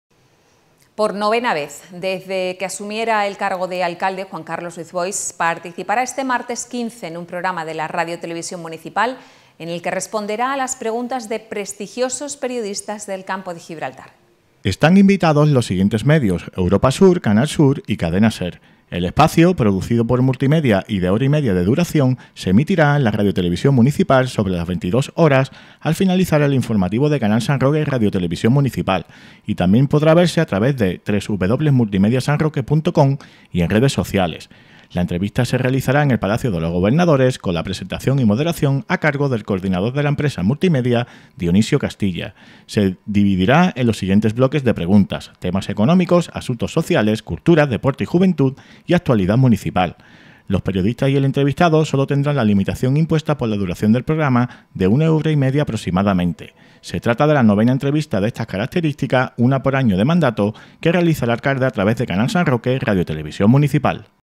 NOTICIA. PREVIA PROGRAMA ENTREVISTA PERIODISTAS A ALCALDE. 14 OCTUBRE 2019
Por novena vez desde que asumiera el cargo de alcalde, Juan Carlos Ruiz Boix participará mañana martes en un programa de la radiotelevisión municipla en el que responderá a las preguntas de prestigiosos periodistas del Campo de Gibraltar.